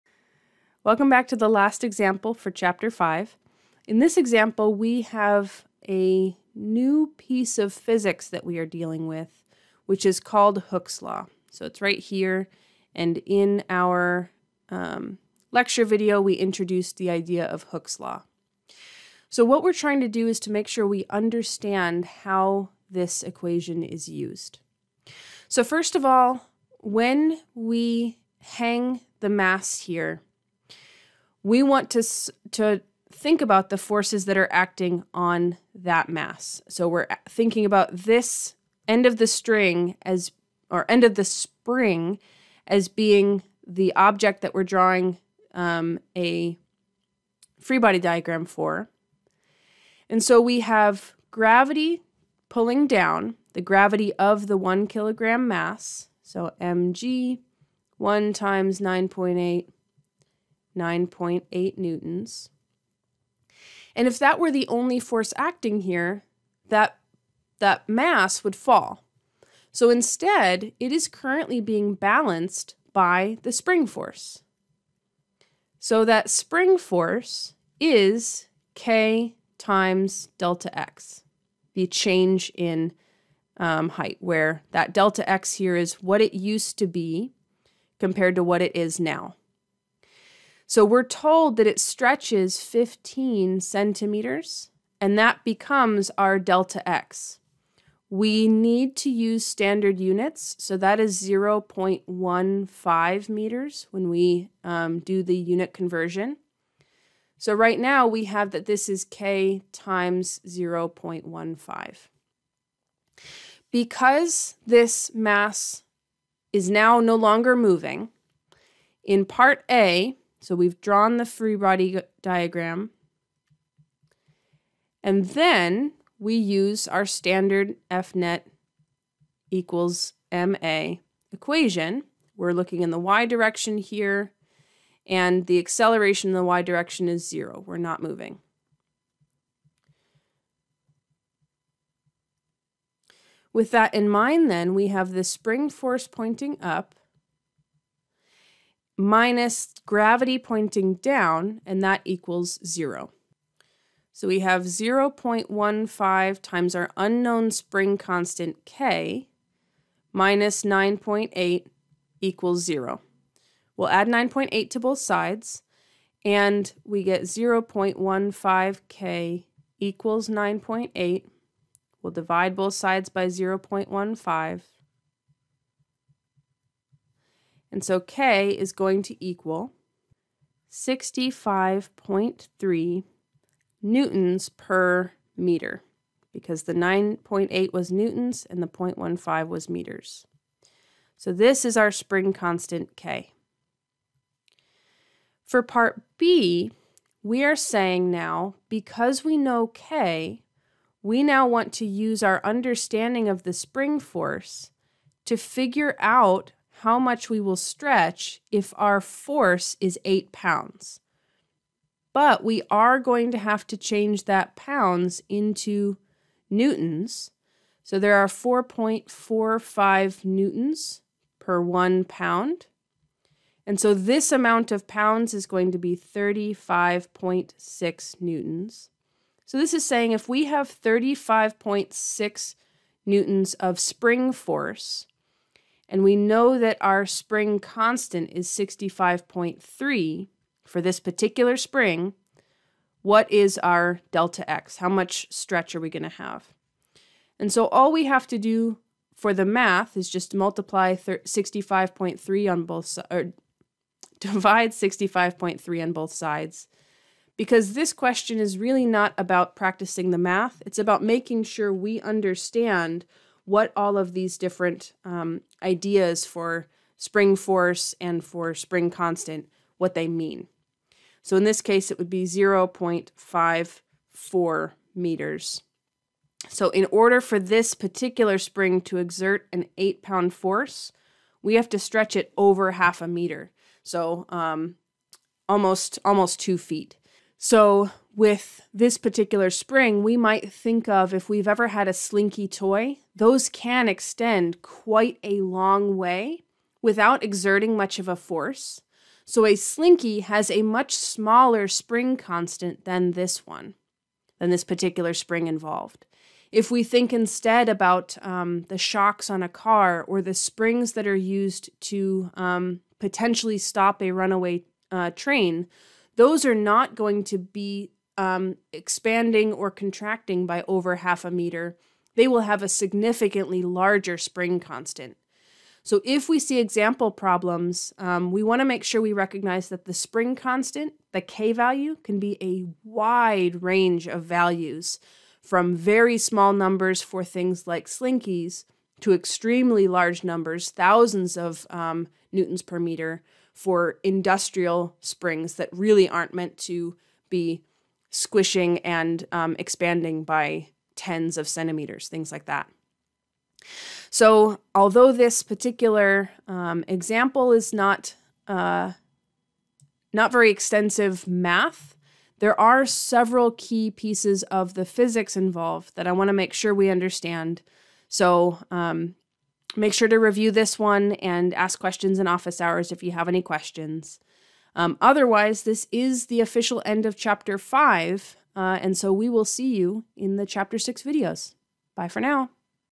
GRCC Physics 125 - Online Lecture - Example 5E
This is an example from Openstax Chapter 5 (Further Forces: Friction, Drag Force, Spring Force). It is intended to be watched after the introductory lecture videos for this chapter.